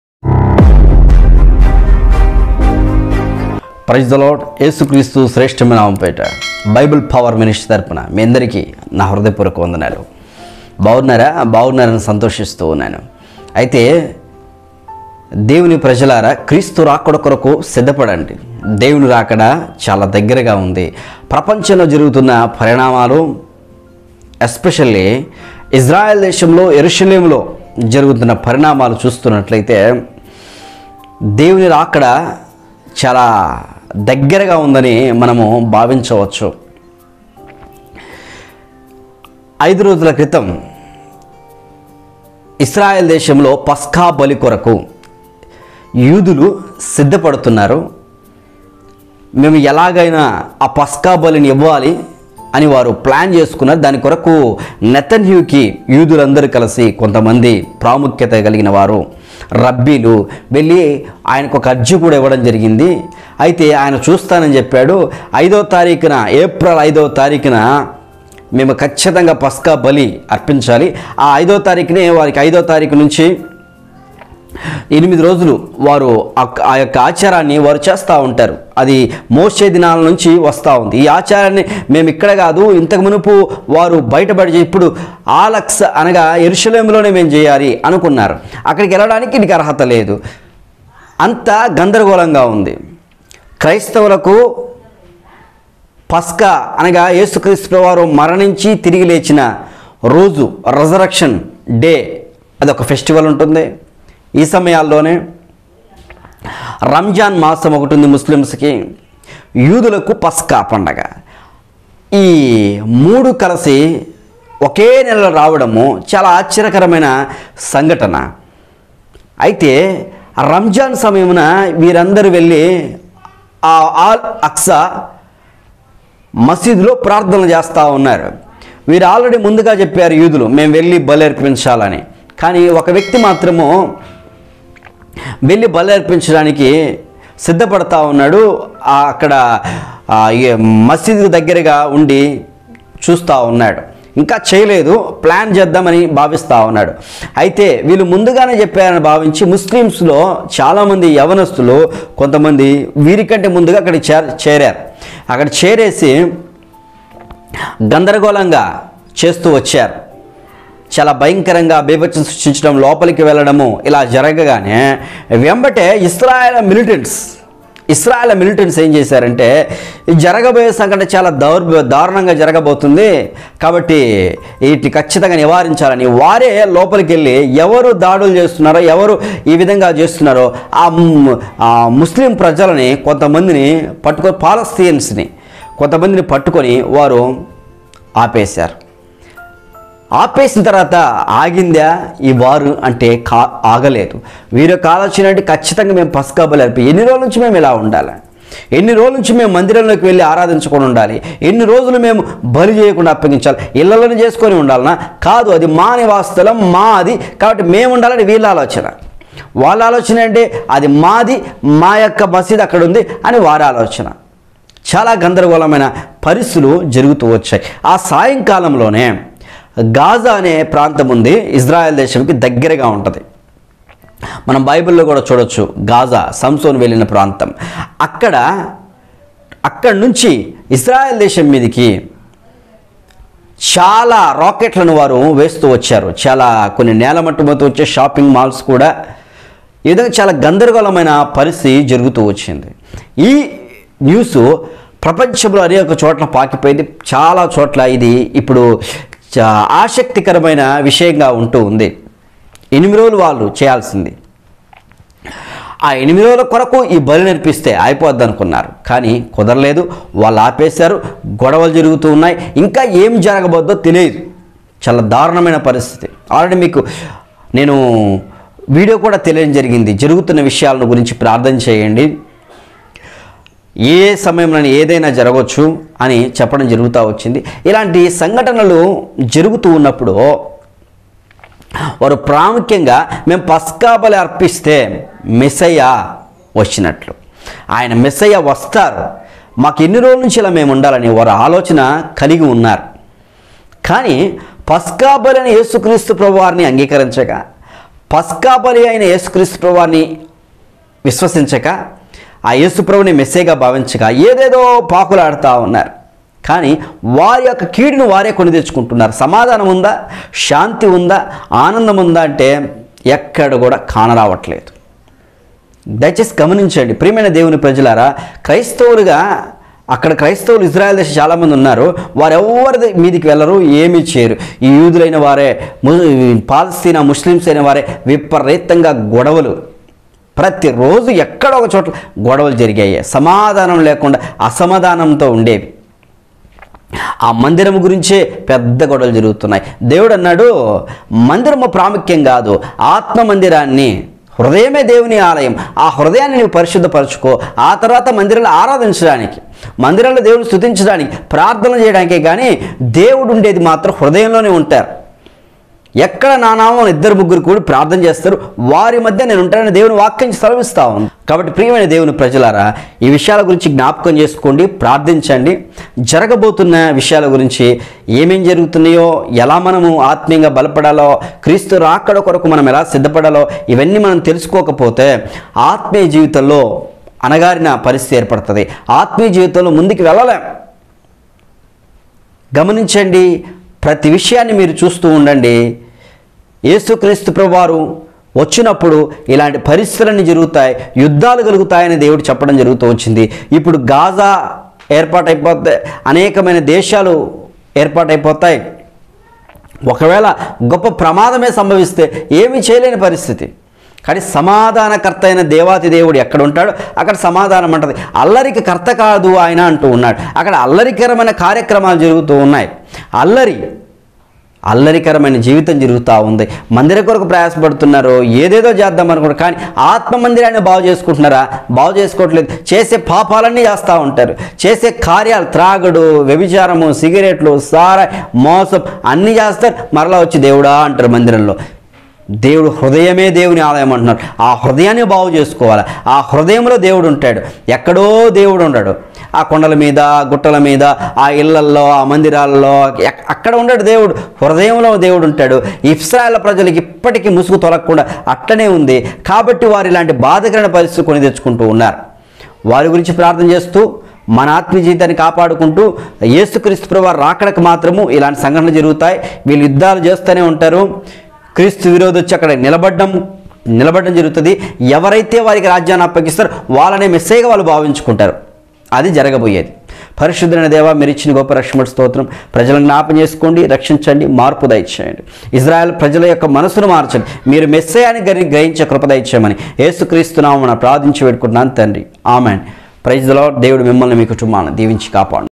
ఇశ్రాయేలు దేశంలో మందిరం దగ్గర ఏప్రిల్ 5న ఏం జరిగింది||పస్కా బలి కొరకు జరిగిన గొడవలు||Israel Passover
దేవుని రాకడ కొరకు సిద్ధపడే సమయాలు ఇవి